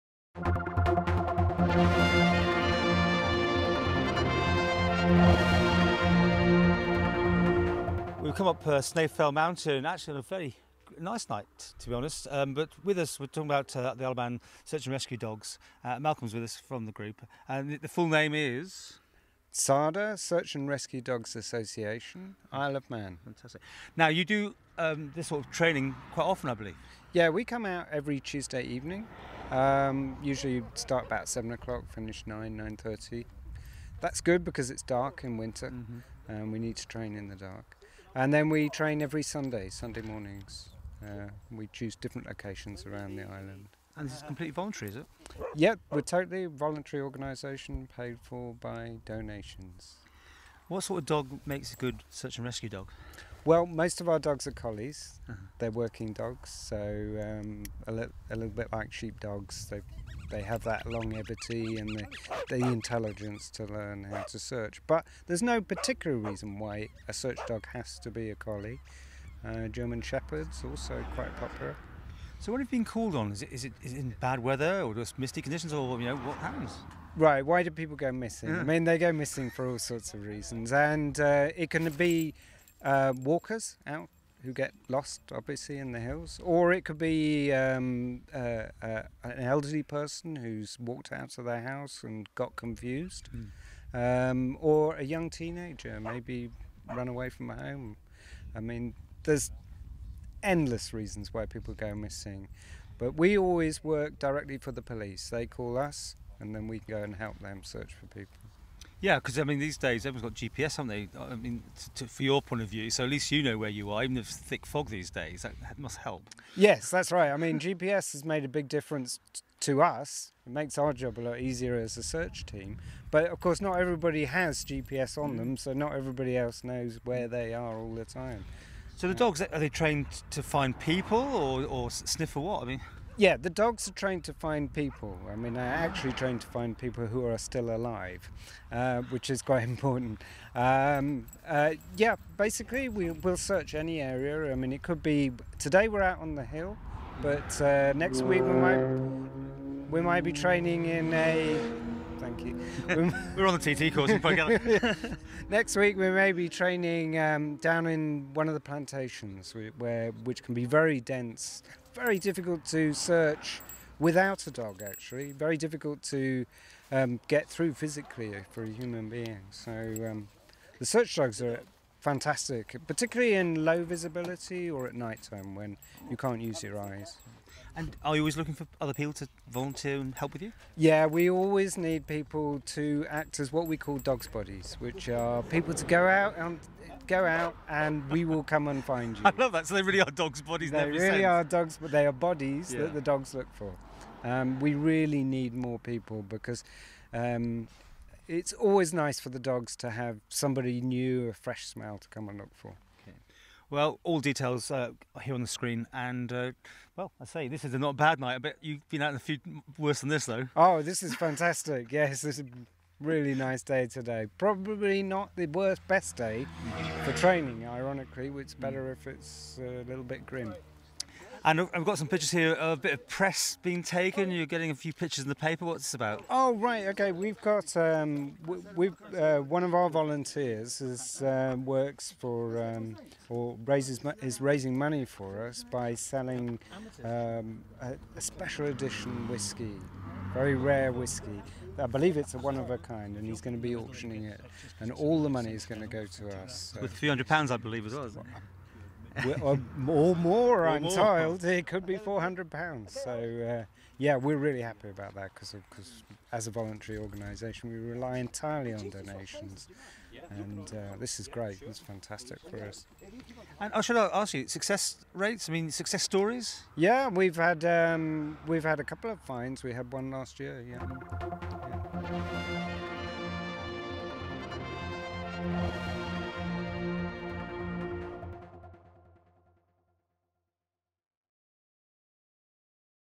IoM TV archive: Search and Rescue dogs: 4.9.2013
Mal Stewart from the Isle of Man Search and Rescue Dog Association talked to Paul Moulton in September 2013 about their organisation.